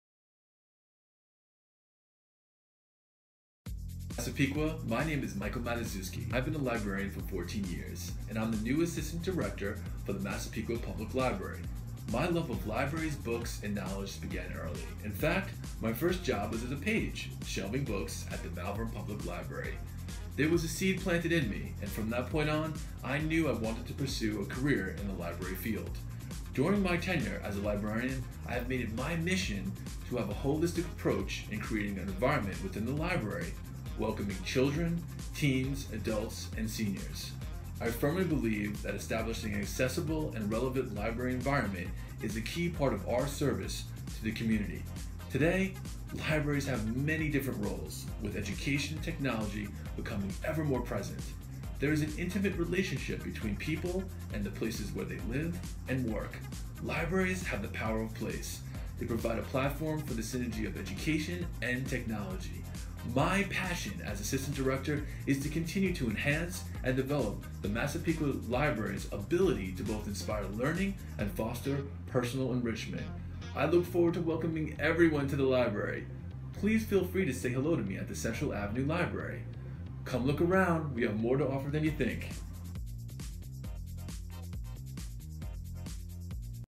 Michael Matuszewski Introduction
Introduction to the Library's Assistant Director, Michael Matuszewski.